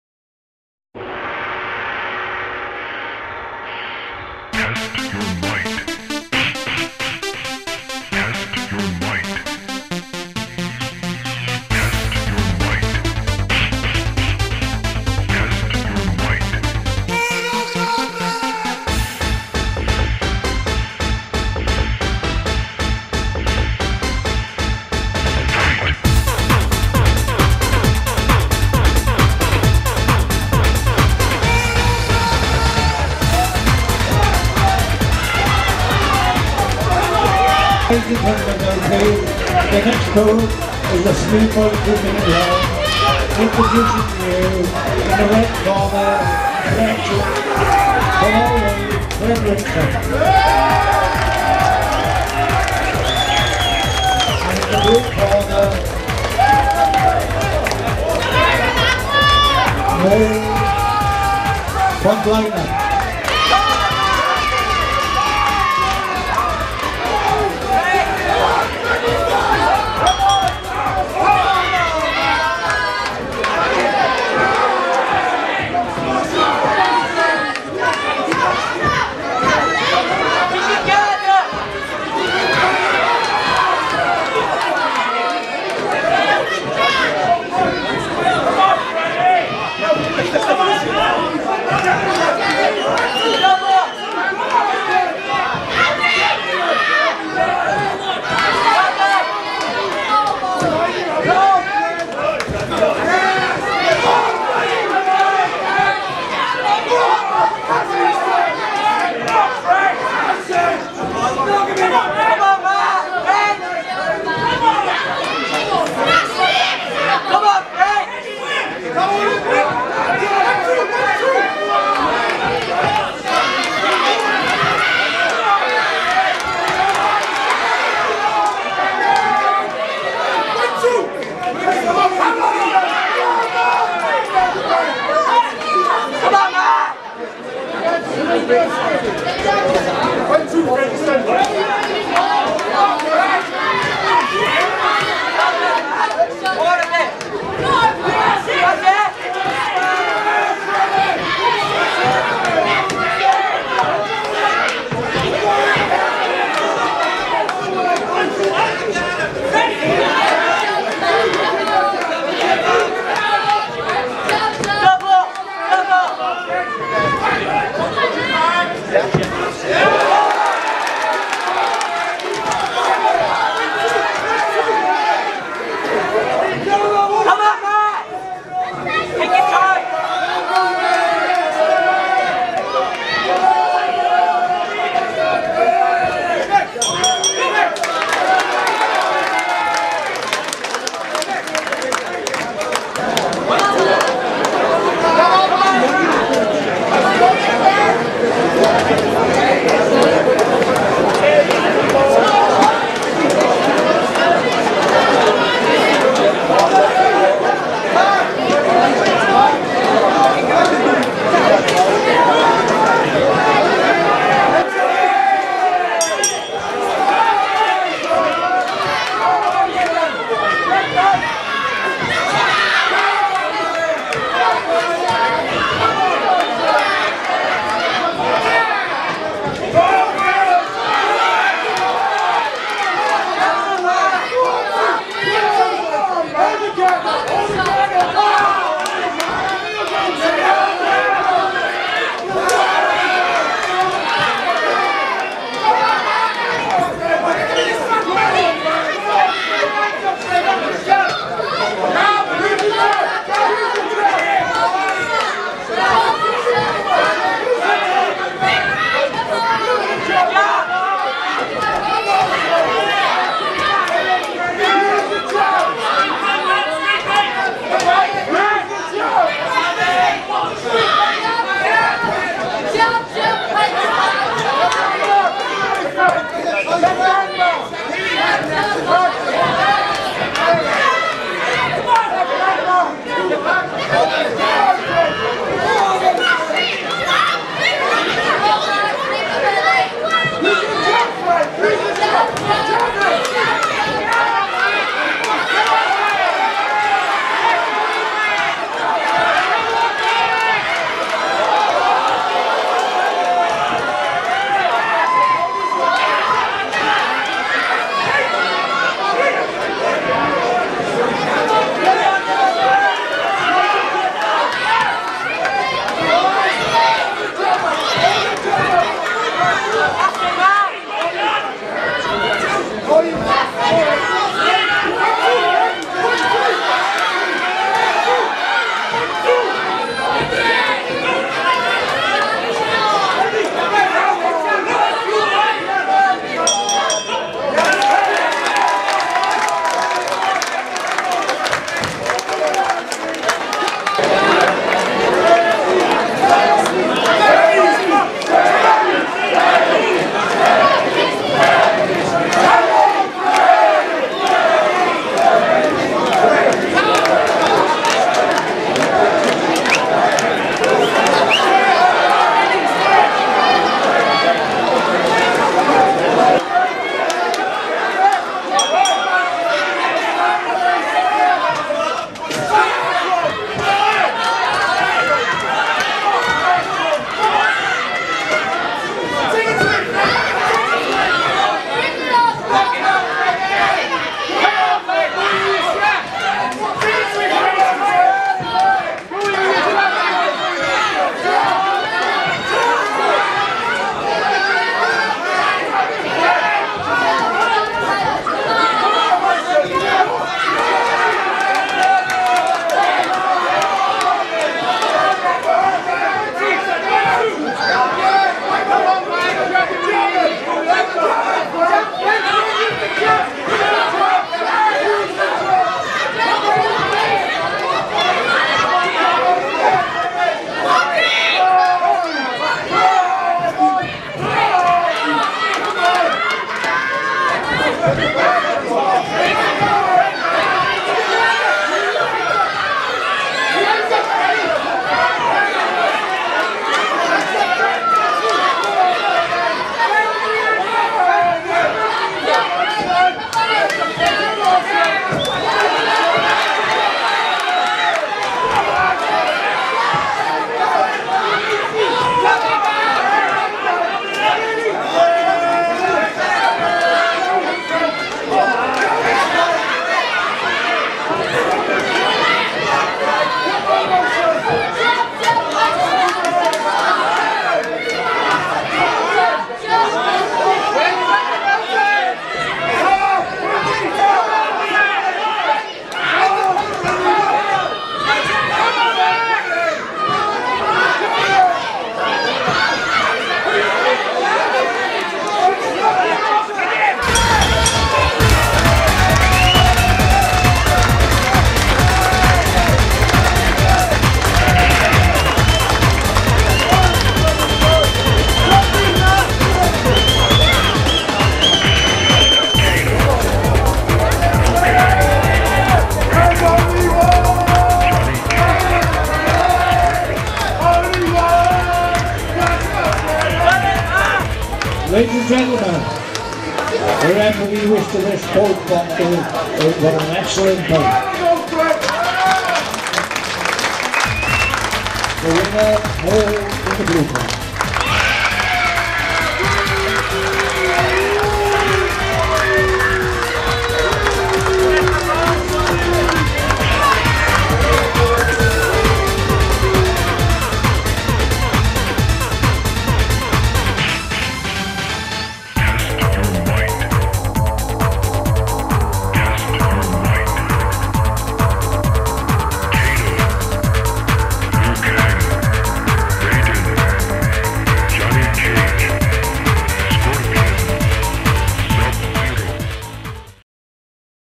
mark fredrickson first fight
my first aba fight hope you enjoy, good fight but fitness let me down